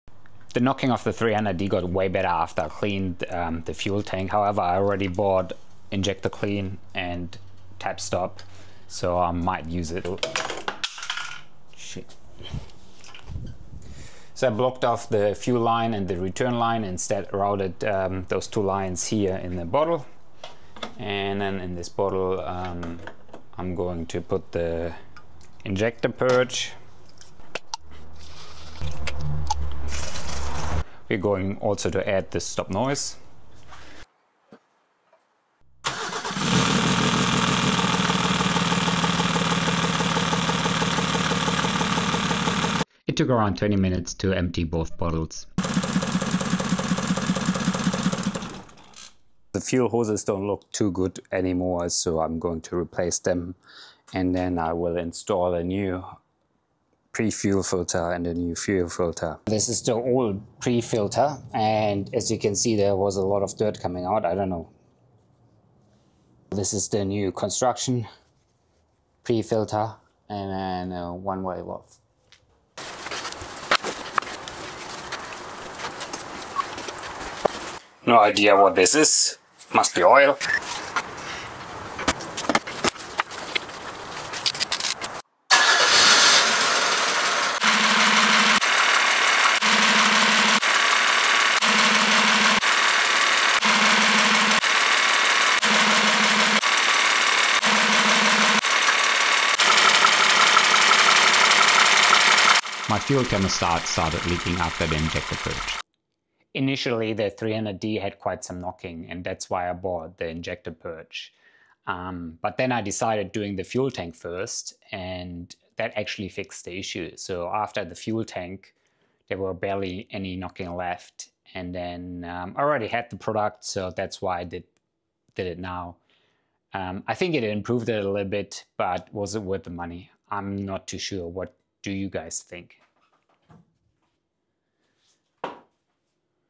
Diesel Injector Purge on Mercedes 300D w124 OM603 | No Diesel, Just Cleaner! + Tap Noise Fix
In this video, I perform a diesel injector purge on my 1986 Mercedes-Benz 300D with the OM603.912 engine - using only injector cleaner, no diesel fuel. I disconnected the fuel feed and return lines and ran the cleaner straight through the injection system. I also added a tap noise additive to the oil to see if it helps quiet down the old diesel engine.

If you’re trying to restore injector spray quality, improve idle, or reduce knocking in an old diesel, this method might help. The fuel thermostat started leaking along the way - watch to the end for results and commentary.

🔧 Tools & products used:
 • Diesel injector cleaner (used undiluted)
 • Oil additive for tap/clicking noise
 • New inline filter and fuel hose

⏱️ Chapters:
0:00 – Intro
0:09 – Preparation & purge setup
0:35 – Running the injector cleaner
0:49 – Fuel line & filter replacement
1:27 – Results after the purge
1:39 – Found a fuel thermostat leak
1:45 – Outro & my thoughts